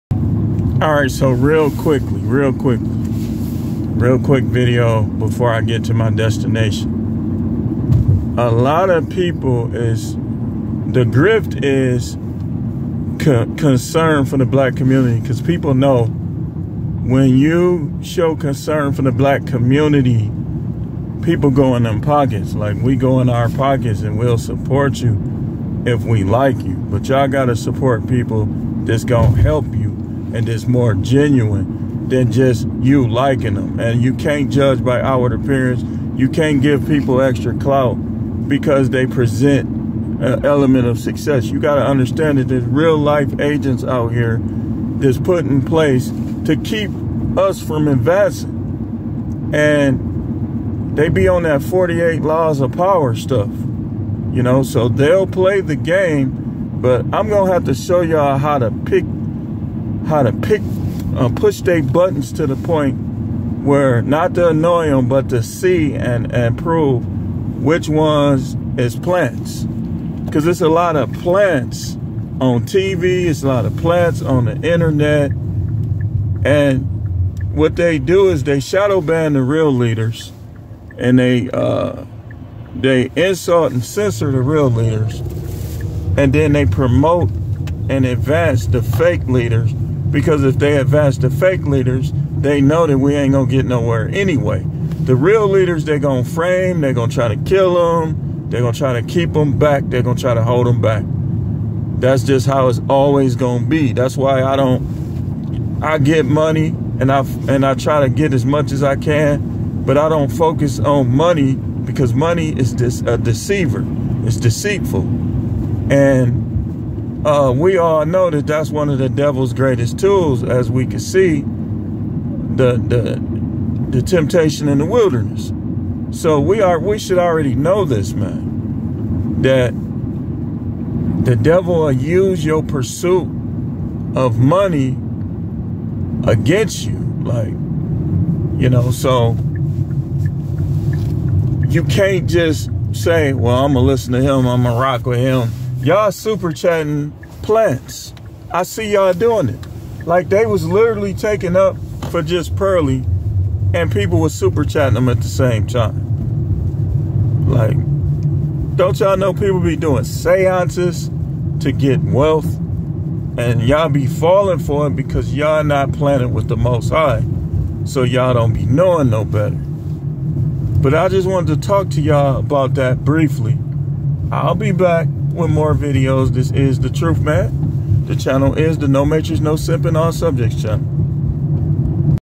Youtube plants posing as leaders in the black community but really r agents/plants/ on 48 lop stuff
This video is about puppet plants in the black community.